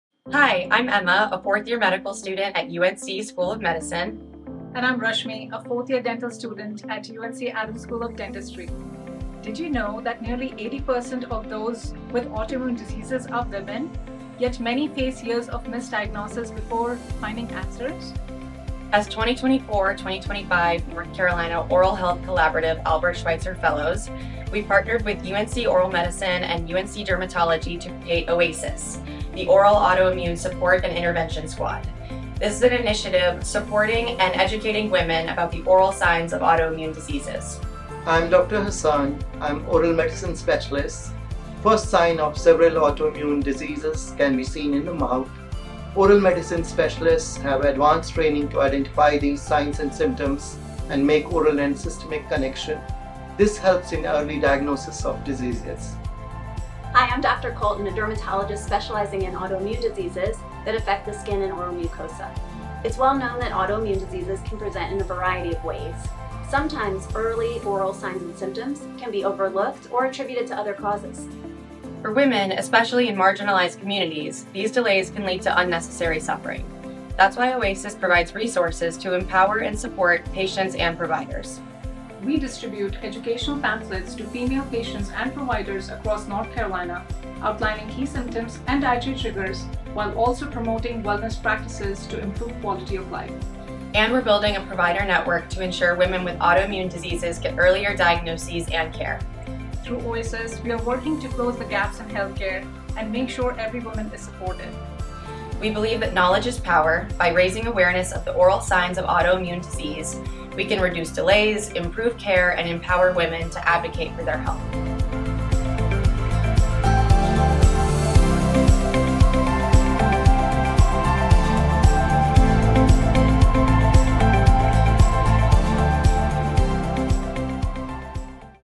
2024 Fellows Emma Myers and Rashmi Deshmukh
The Fellows led an nterdisciplinary initiative created to improve the early recognition, diagnosis, and quality of life for women experiencing oral symptoms of autoimmune disease. Through weekly clinics at UNC Dermatology, the Fellows identified and supported over 100 patients, many of whom had previously gone years without a diagnosis.